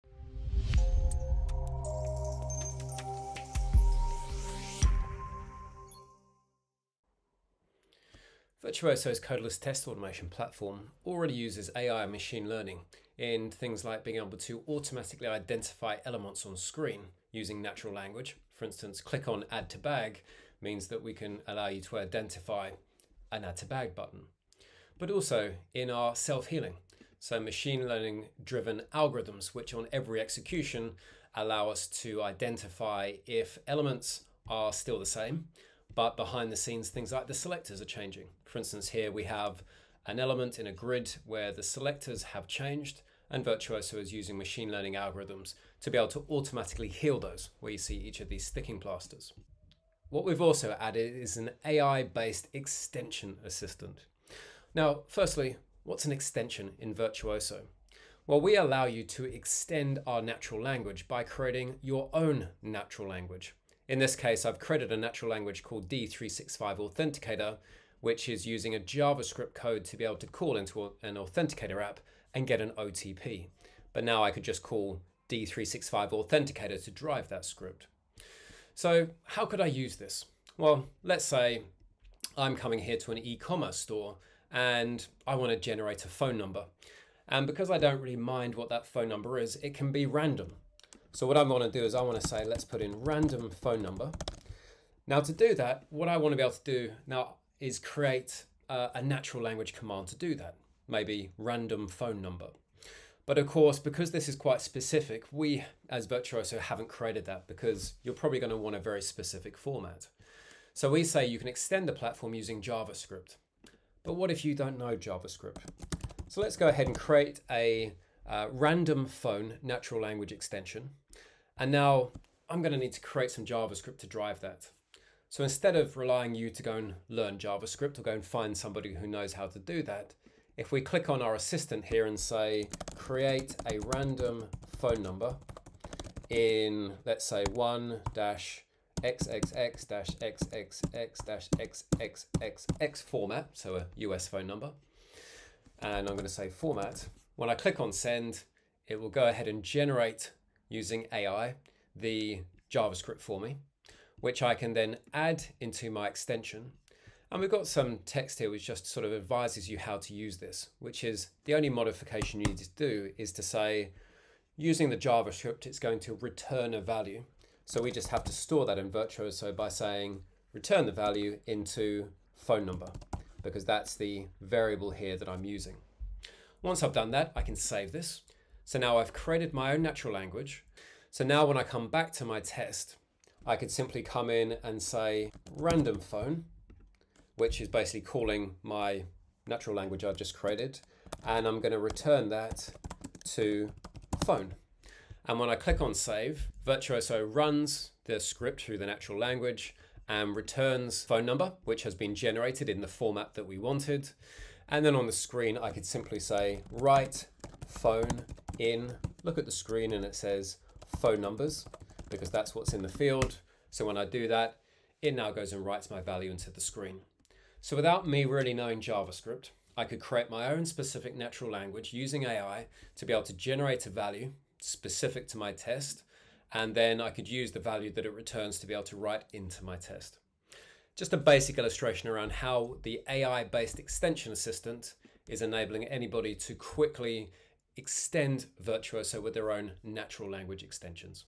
Virtuoso Capabilities - AI-Based Extension Assistant
In Virtuoso we trust in no-code test automation, however, there are times when one needs to use JavaScript, for example, to create a Virtuoso extension. The good news is that now one of our bots at Virtuoso will be available to help you during the coding of your extensions. Just explain what you wish to achieve with your extension and our bot will use its AI capabilities to suggest a possible solution for it